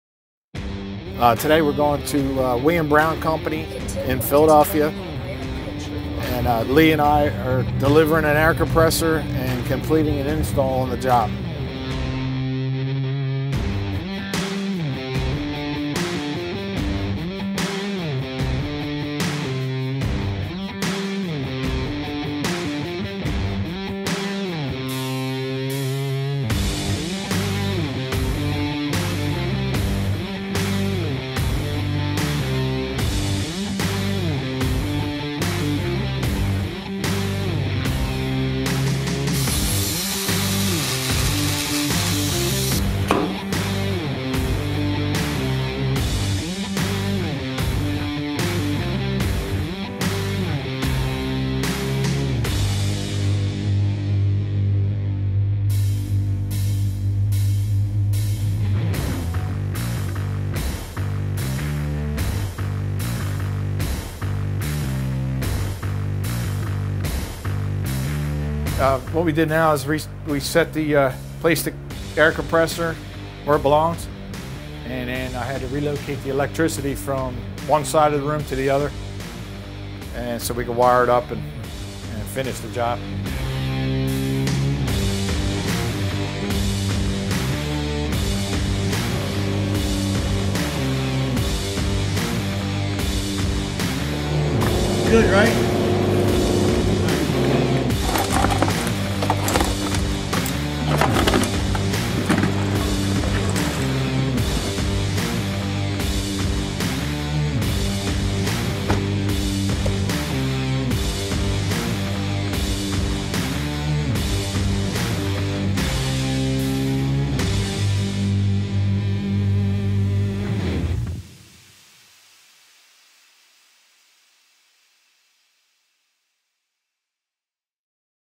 Day in the Life | Kaishan 7.5hp Rotary Screw Compressor Install 2020
Mike and Lee installed a Kaishan KRSB 7.5hp Rotary Screw Compressor at the William Brown Co. in Philadelphia.